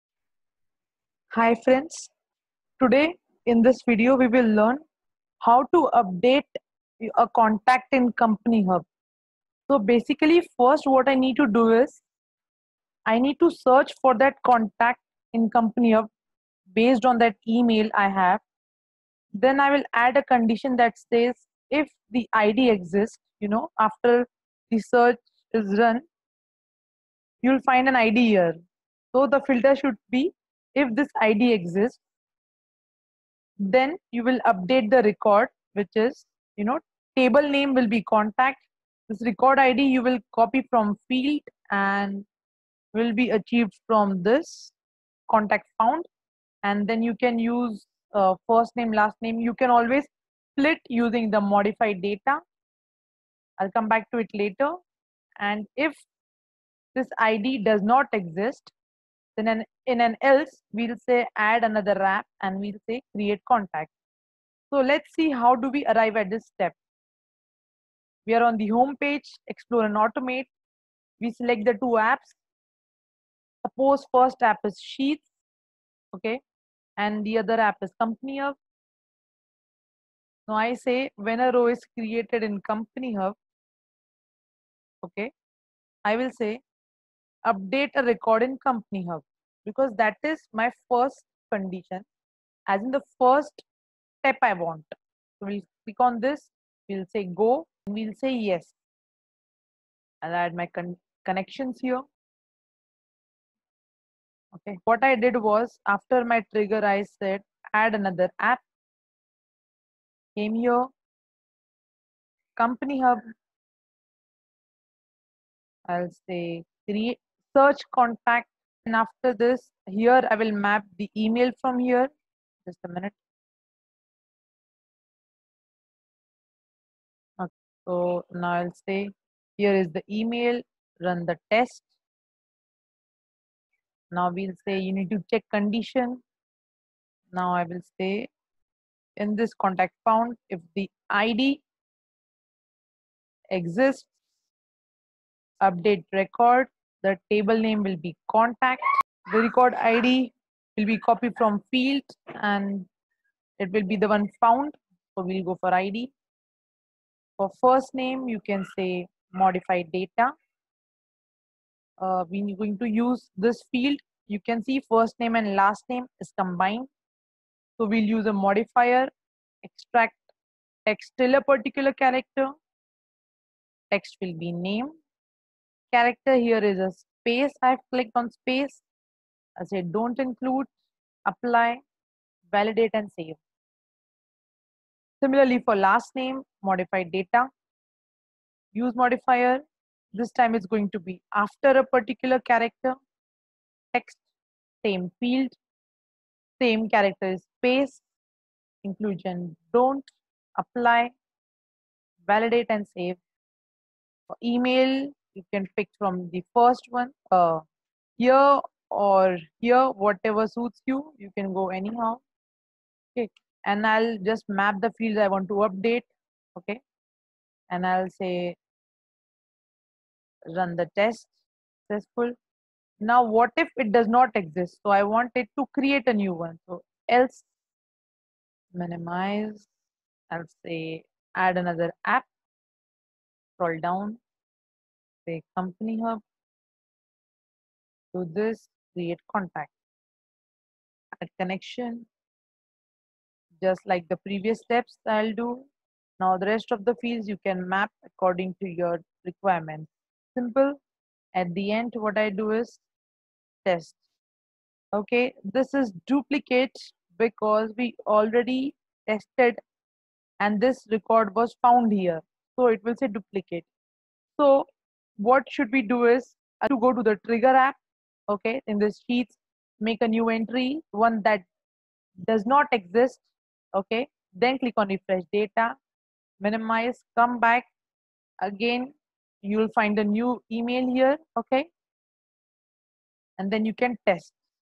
Search - update or create contact in CompanyHub
This video helps you build a flow wherein you first search for a contact (based on email) in CompanyHub. Then update (if already exists) or create a new contact (if it doesn't exist already) in CompanyHub.